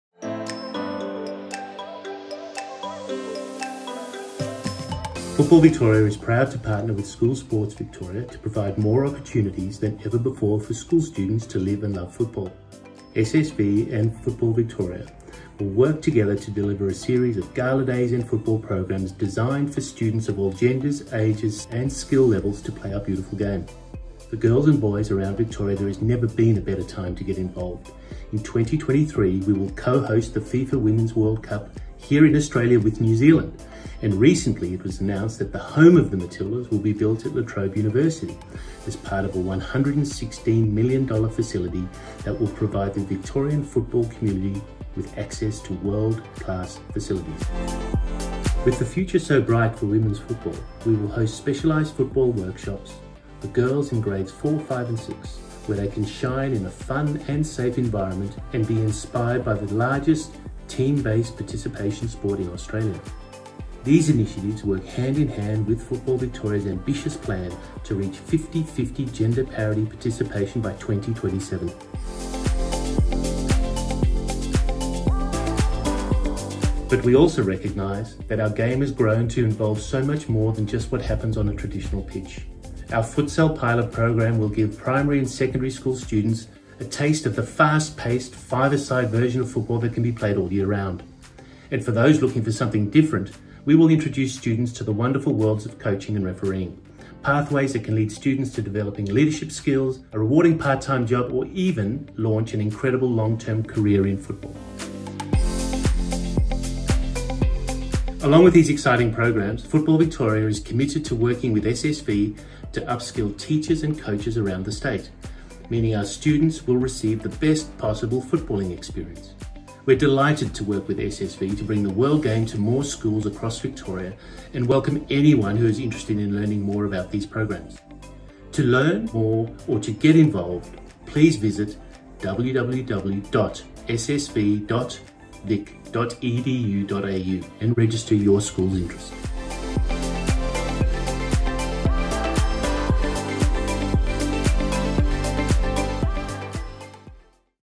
FV and School Sport Victoria unite to bring more football and Futsal to schools across the state
Football Victoria is proud to partner with School Sport Victoria to provide more opportunities than ever before for school students to live and love football.

Three tailor-made programs, including Futsal are set to kick-off in Term 4 which will are designed for students of different ages and abilities to get involved in the world game.